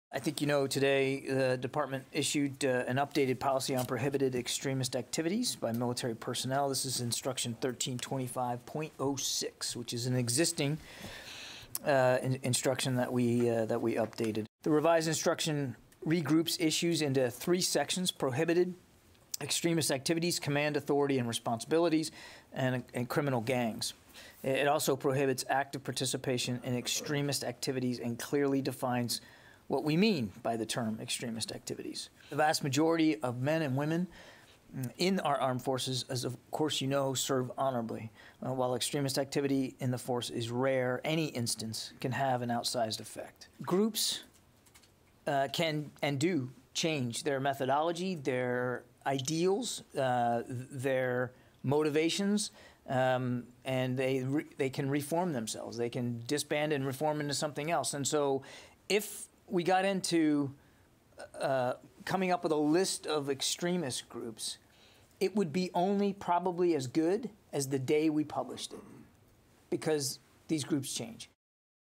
Pentagon stops short of banning military from extremist groups
The Pentagon stopped short of creating a list of extremist groups that military members cannot join.